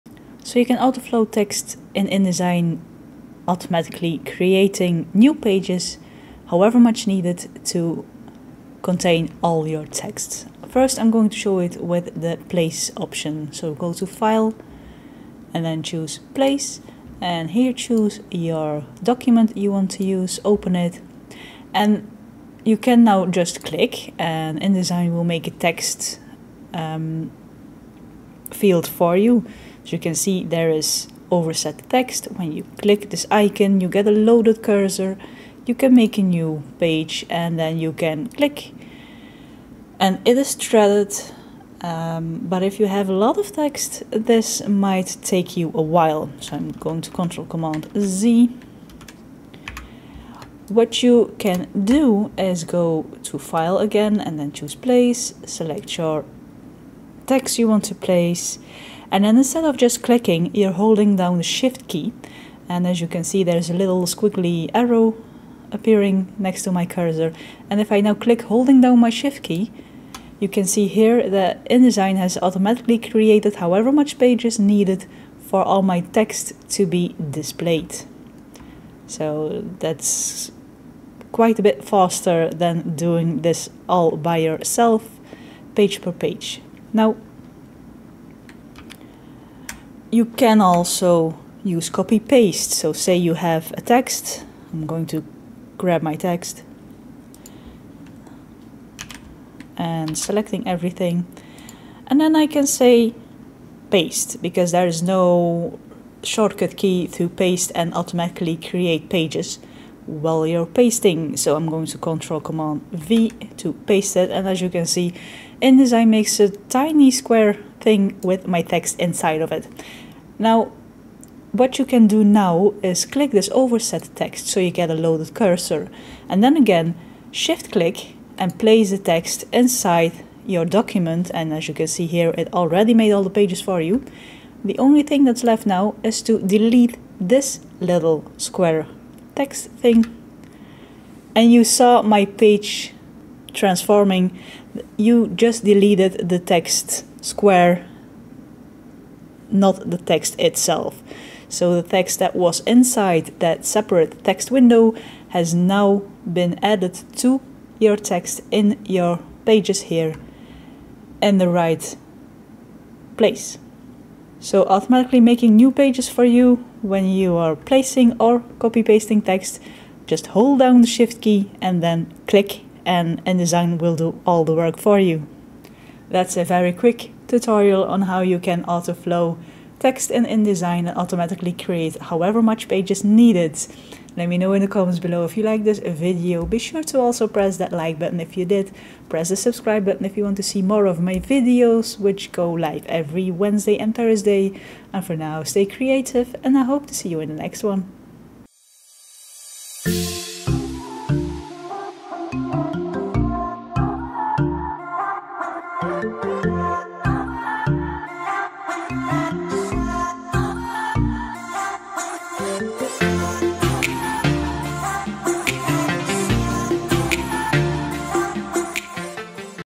How to Autoflow Text and Add Pages Automatically [Adobe Indesign]
In this tutorial I will show you how you can autoflow text (thread text) and automatically add pages in Adobe Indesign.

Editing software: Adobe Premiere Pro and Adobe Photoshop
Editing: Adobe Premiere Pro
Intro & Outro: Adobe After Effects